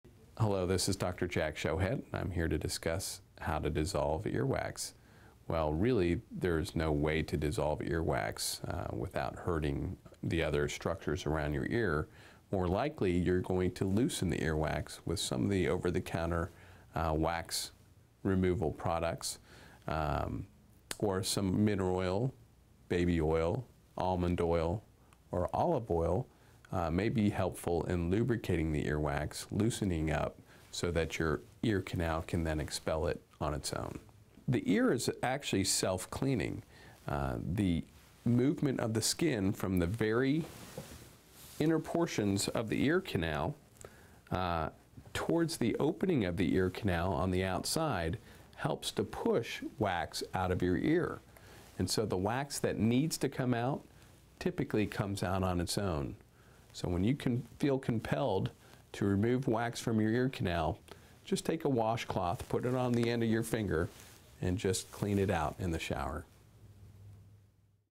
Earwax Removal : How to Dissolve Earwax
Dissolving earwax is not possible without harming the surrounding components of the ear, but loosening up earwax buildup can be done with an over-the-counter product or with an oil. Loosen up earwax before having it removed by a doctor with information from a practicing otologist in this free video on ear health.